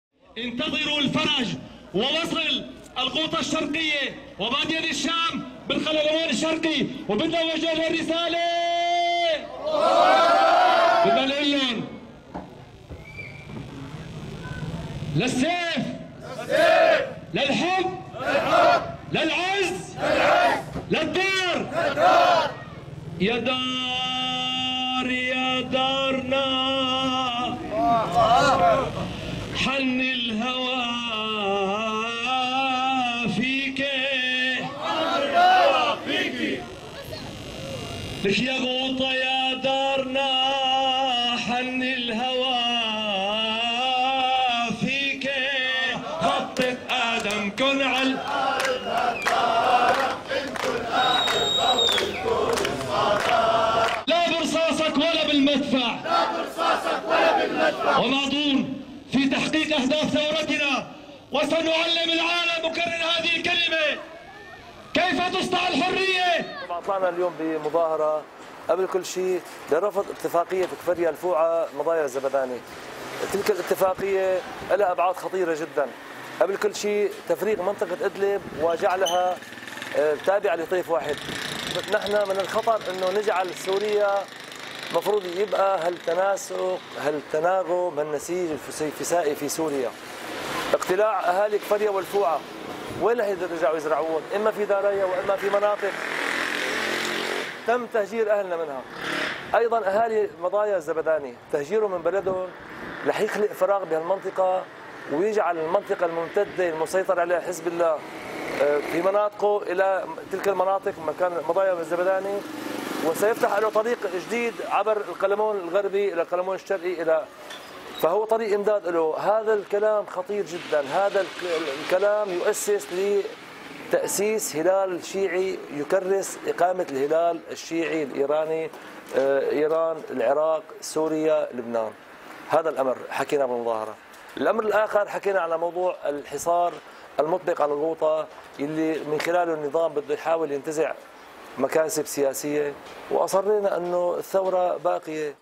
Rif Dimashq: Protest in Douma city to denounce Kafraya and Fua agreement 31-3-2017
Protest broke out on Friday after the prayers in Douma city, Rif Dimashq province, urging to break the siege over Ghouta area and denounce Kafraya, Fua, Madaya and Zabadany agreement
مظاهرة في مدينة دوما تطالب بكسر الحصار عن الغوطة وتندد باتفاق الزبداني  فوعة